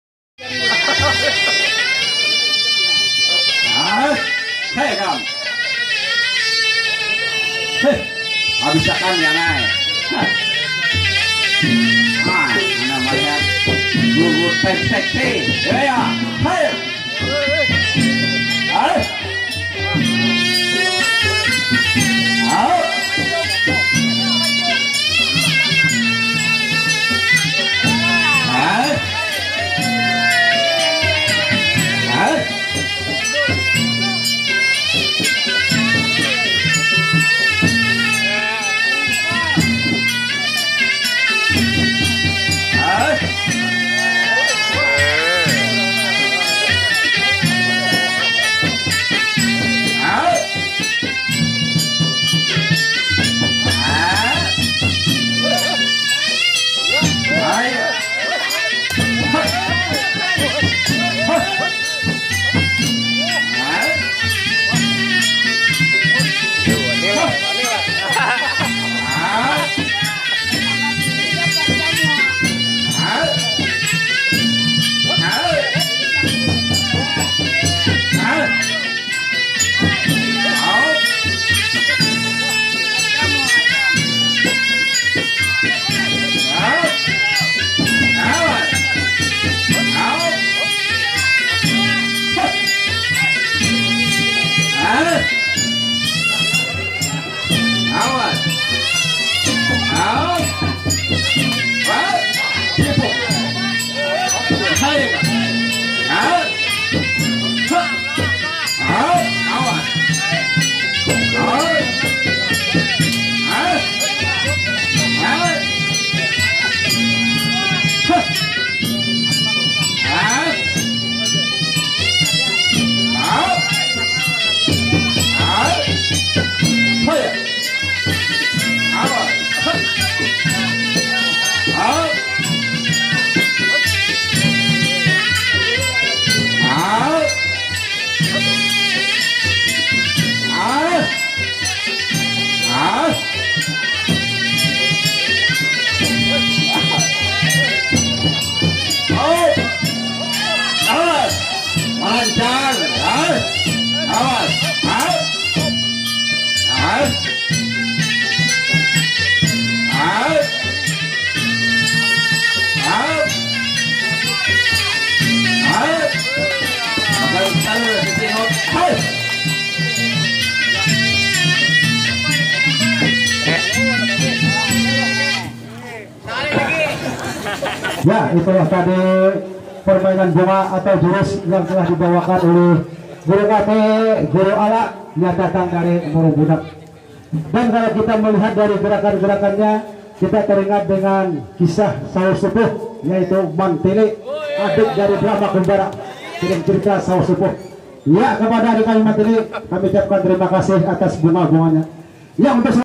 Gadis Cantik Sangat Mahir Bermain Silat Kuntau
Paras cantik seorang gadis yang satu ini membuat mata para lelaki tertuju padanya, namun jangan sekali sekali menggoda atau berbuat jahat kepadanya, bisa bisa Anda babak belur dibuatnya. Karena gadis yang satu ini salah satu pewaris jurus seni bela diri kuntau yang terkenal di daerah Hulu Sungai Kalimantan Selatan.